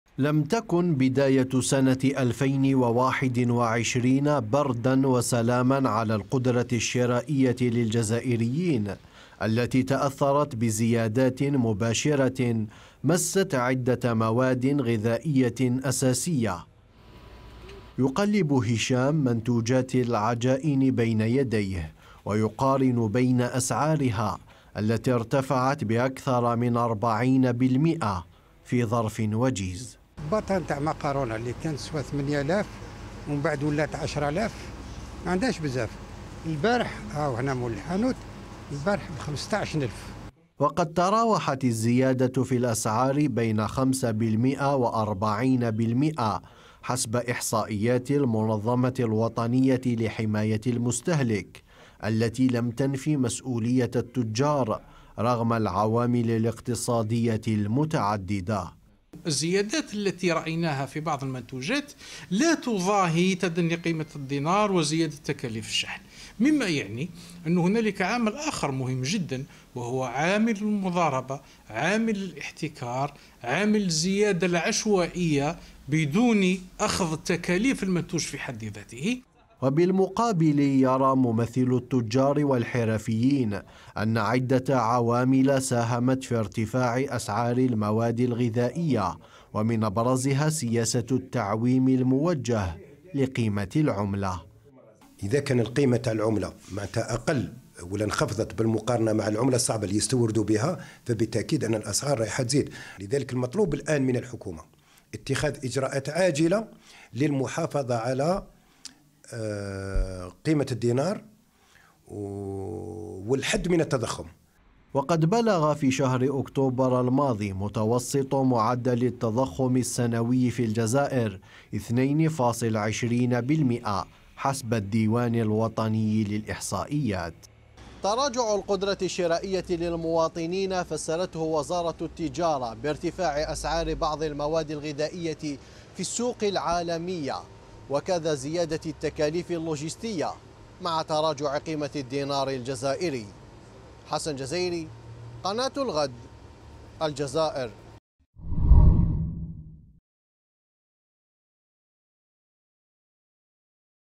الجزائر | ارتفاع أسعار المواد الغذائية في البلاد واتهامات للتجار بالمضاربة والاحتكار
ارتفعت أسعار المواد الغذائية في الجزائر ما بين خمسة في المئة وأربعين في المئة بحسب إحصائيات المنظمة الوطنية لحماية المستهلك التي اتهمت التجار بالمضاربة والاحتكار، فيما نفت جمعية التجار والحرفيين الاتهامات، مؤكدة أن انخفاض قيمة الدينار الجزائري مقابل اليورو والدولار من أبرز العوامل التي ساهمت في ارتفاع الأسعار... المزيد من التفاصيل عبر التقرير المصور لمراسل الغد من الجزائرحسان جزايري، عبرالفقرة الإخبارية.